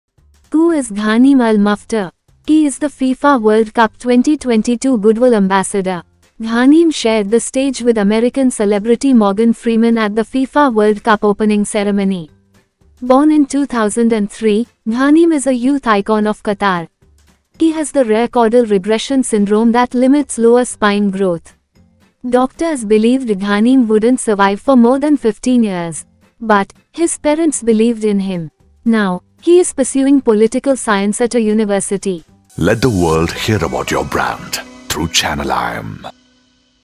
Meet Ghanim Al Muftah who co-hosted the Qatar World Cup Opening Ceremony alongside Morgan Freeman
Who is Ghanim Al Muftah?
He is the FIFA World Cup 2022 Goodwill Ambassador
Ghanim shared the stage with American celebrity Morgan Freeman at the FIFA World Cup opening ceremony
Born in 2003, Ghanim is a youth icon of Qatar
He has the rare Caudal Regression Syndrome that limits lower spine growth
Doctors believed Ghanim wouldn't survive for more than 15 years
But, his parents believed in him
Now, he is pursuing political science at a university
He is an entrepreneur, activist, social media influencer and sports lover